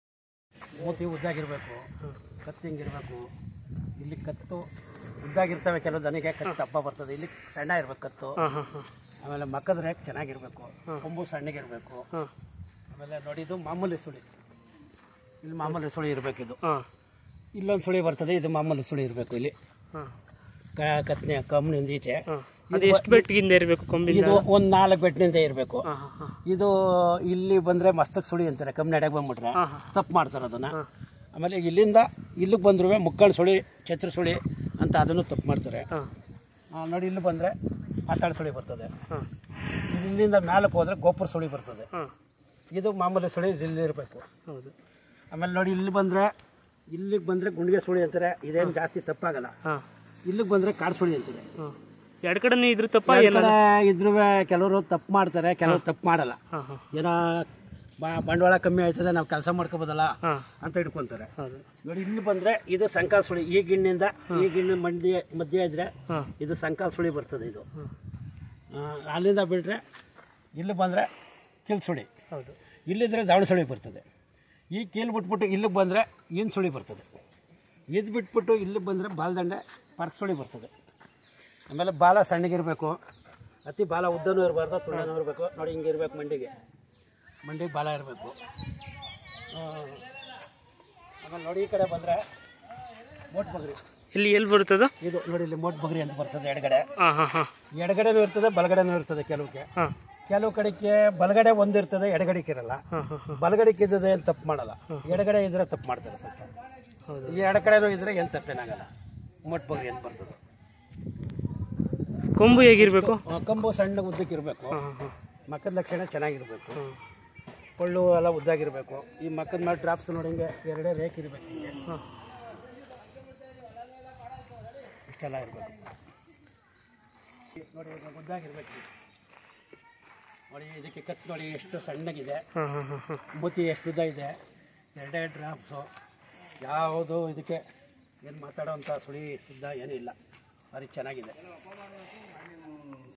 Experienced Hallikar stud bull breeder explaining about hair whorls (suli) in Hallikar breed.
ಹೋರಿ ವೀರಭದ್ರಯ್ಯ ಅವರು ಸುಳಿಗಳ ಬಗ್ಗೆ ವಿವರಿಸುತ್ತಿರುವುದು.